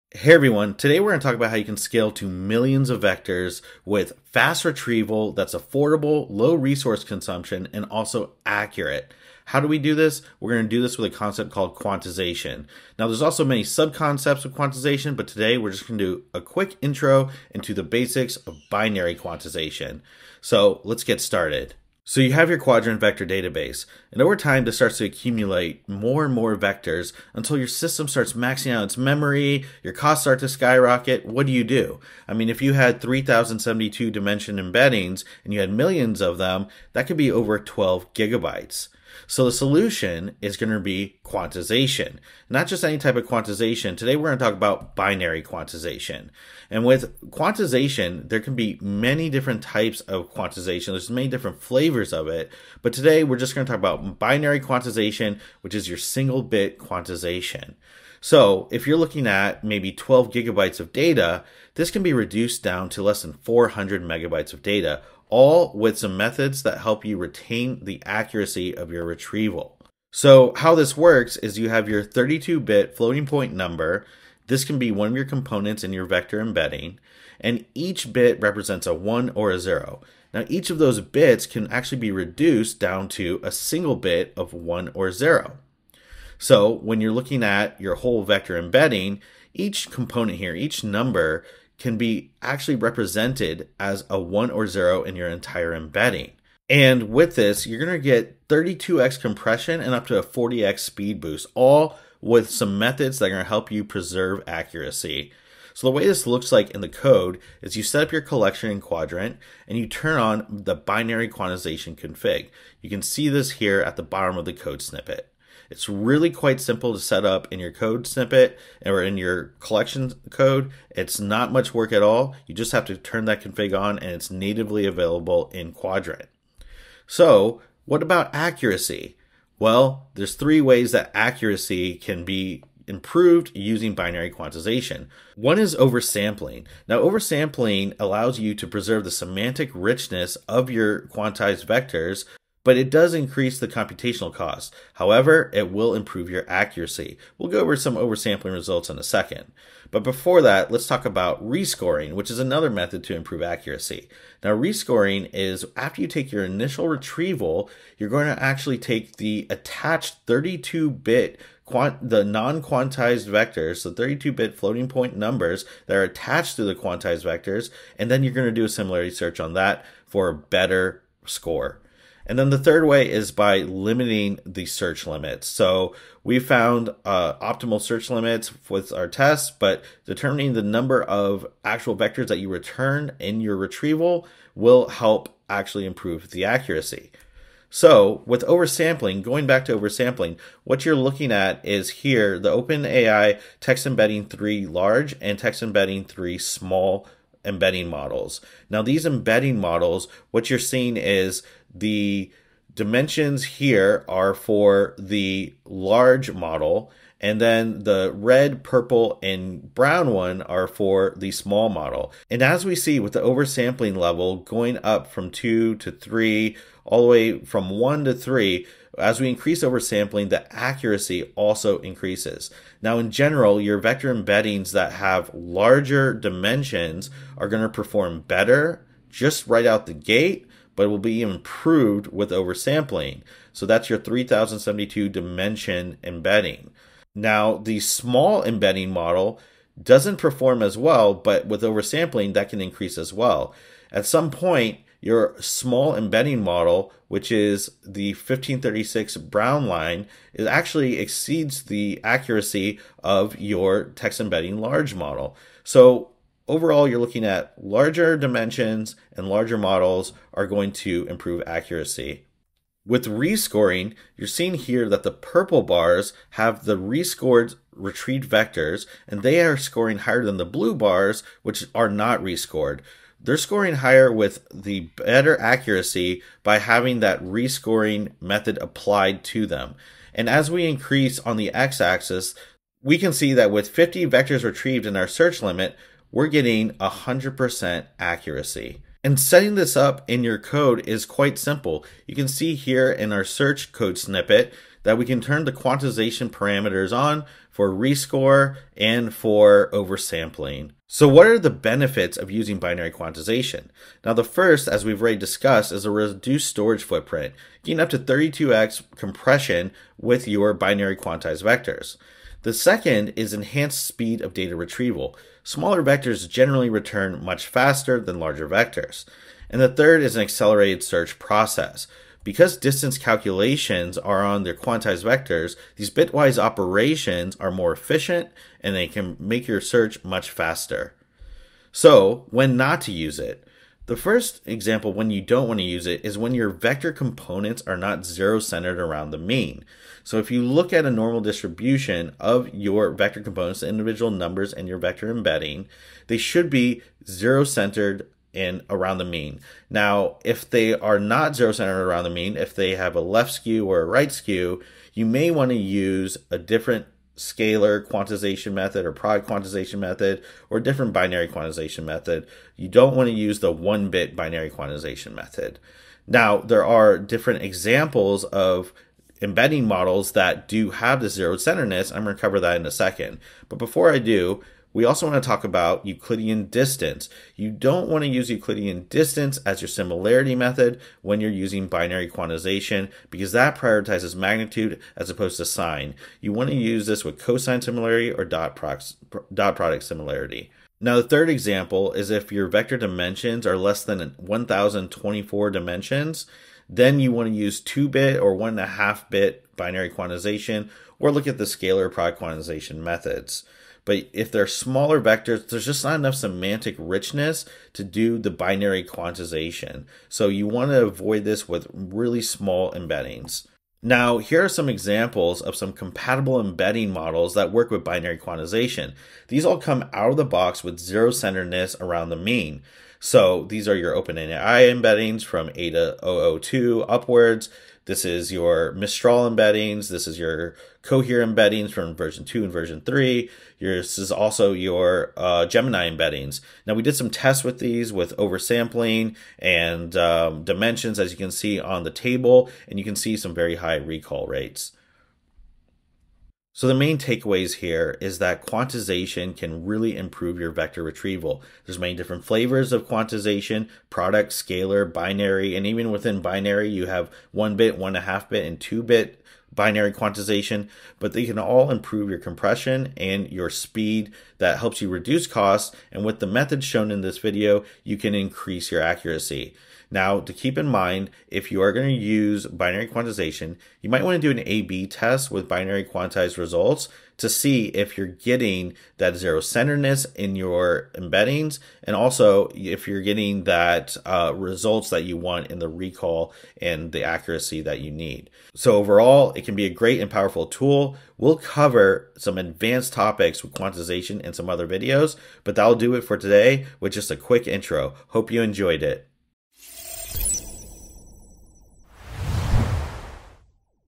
FAST and CHEAP Vector Retrieval with Accuracy | Qdrant Binary Quantization
What if you could scale to MILLIONS of vectors and not sacrifice speed or accuracy? What if this actually reduced your memory footprint?

That is what Qdrant's Quantization Methods allow you to do. In particular, Binary Quantization.

Reduce 12GB of data to less than 400MB while retaining high accuracy and lowering costs.

This video is a quick intro to 1-bit binary quantization, with more to follow on other quantization techniques.